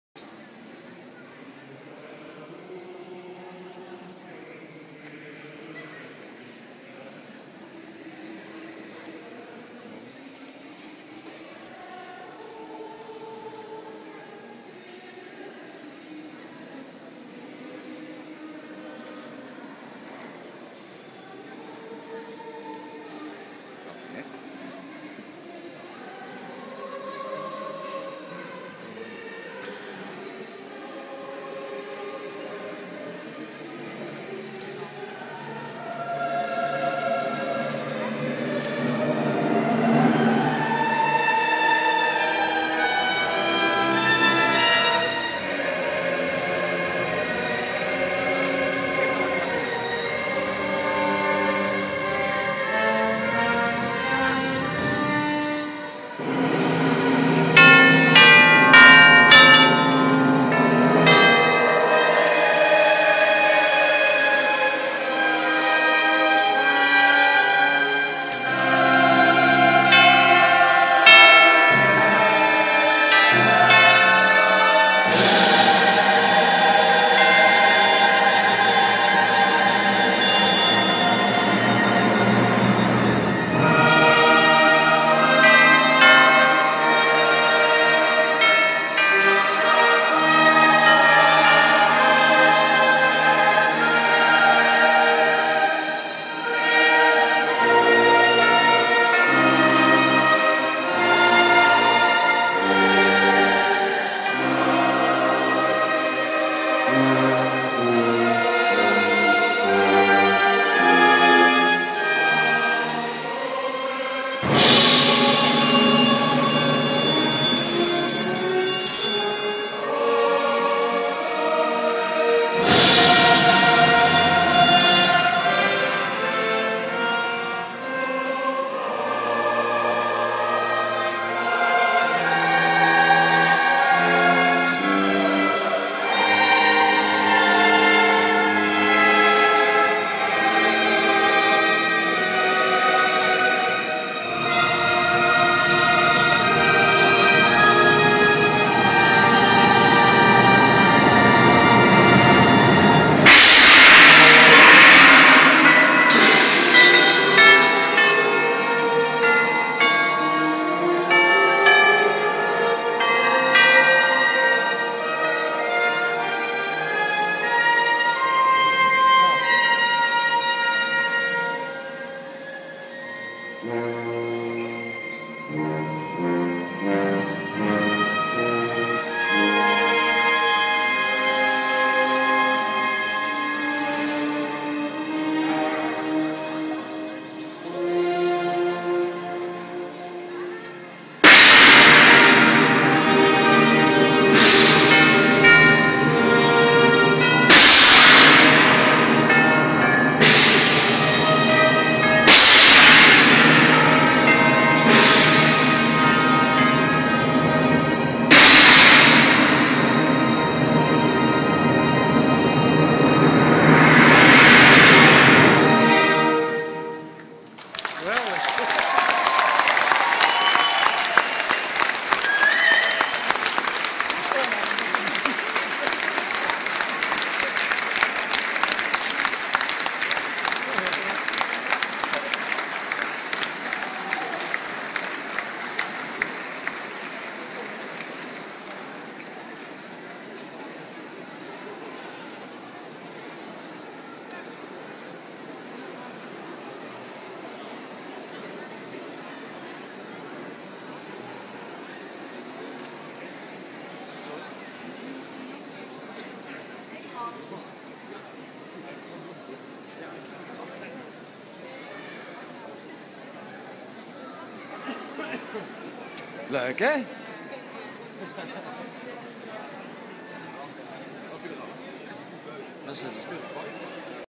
Flashmop HEUVEL Eindhoven
Echt gaaf! Ineens 250 muzikanten van Philips Symfonie Orkest uit het niets tevoorschijn! Kippenvel!